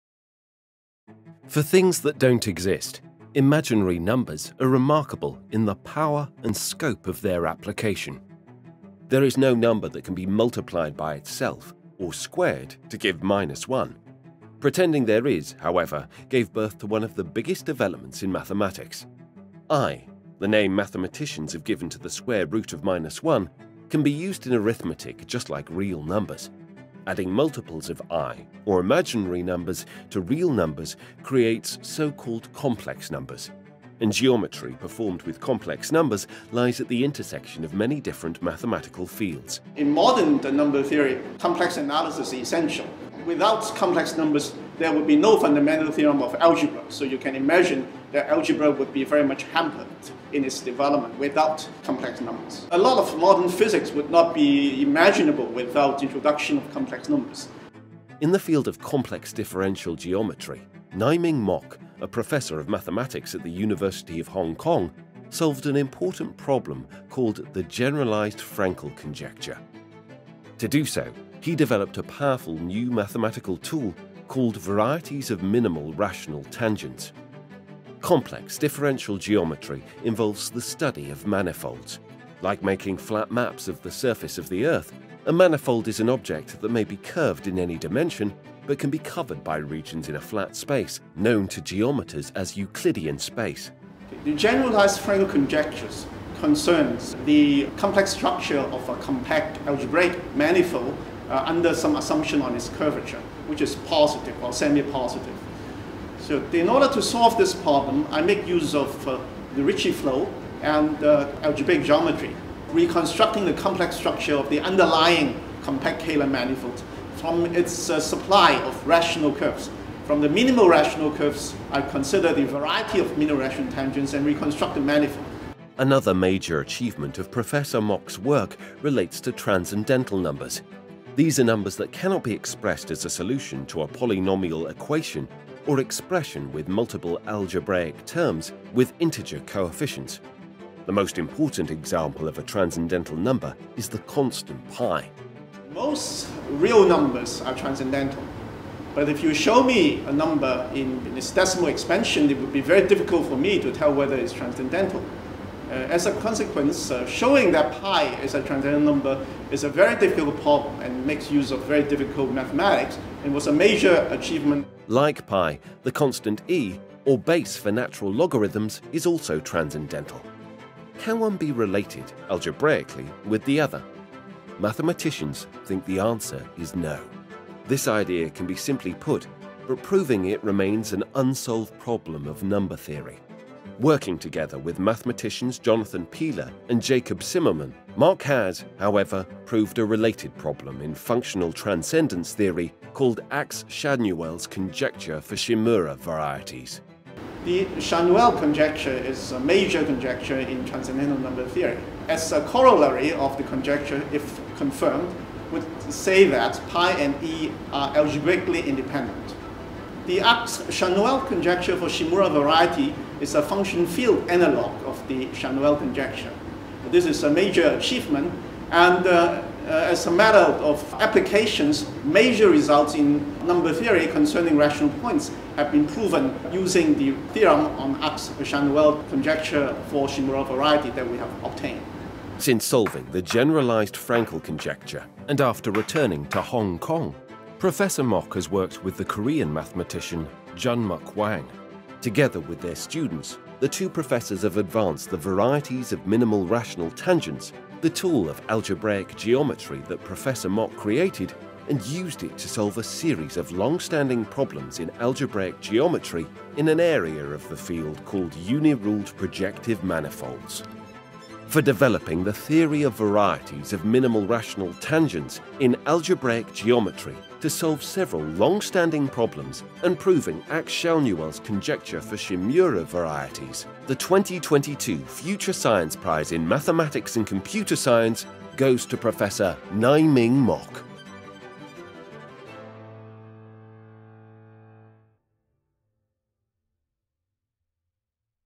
The Research Breakthrough behind 2022 Future Science Prize in Mathematics and Computer Science
Professor Ngaiming MOK was recently awarded the 2022 Future Science Prize - Mathematics and Computer Science Prize for his developing the theory of Varieties of Minimal Rational Tangents in algebraic geometry to solve several long-standing problems and proving Ax-Schanuel’s conjecture for Shimura varieties. Professor Mok is currently the Edmund and Peggy Tse Professor in Mathematics at The University of Hong Kong, Director of the Institute of Mathematical Research. Let’s take a look at his research breakthrough on the theory.

Credits: Future Science Prize Foundation and Hong Kong Academy of Sciences

香港大學謝仕榮衛碧堅基金教授（數學）暨數學系講座教授、數學研究所所長莫毅明教授榮獲「2022未來科學大獎—數學與計算機科學獎」。他創立了極小有理切線簇（VMRT）理論並用以解決代數幾何領域的一系列猜想，以及對志村簇上的 Ax-Schanuel 猜想的證明。齊來深入了解此理論的重要性。

影片鳴謝: 未來科學大獎委員會及香港科學院